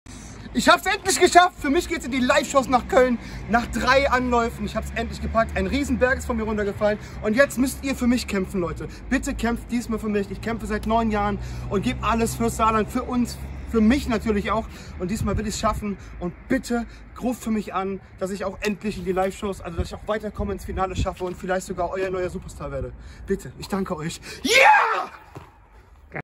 Kevin Jenewein nach seinem Einzug in die Live-Shows bei DSDS
Kevin Jenewein aus Sulzbach hat es bei der Castingshow Deutschland sucht den Superstar (DSDS) im dritten Anlauf in die Live-Sendungen geschafft.